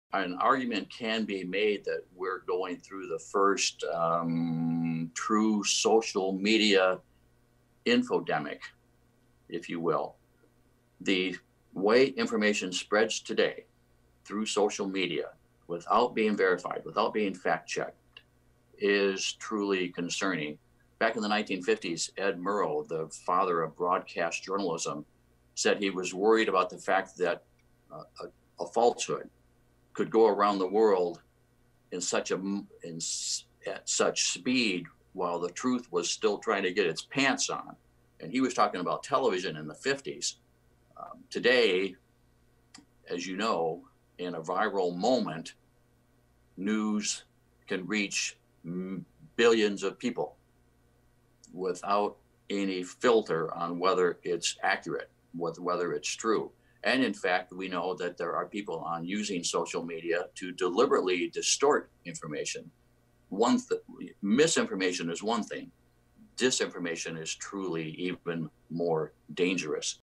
Oakland University Journalism Professor Says Media is Experiencing an "Infodemic"
Gary Gilbert, Oakland University Journalism Program  Director says that journalism has always been evolving, but that the social media age and the misinformation that comes with it has the industry and the consumers experiencing an "infodemic" as they attempt to grasp truth from untruth in daily reporting.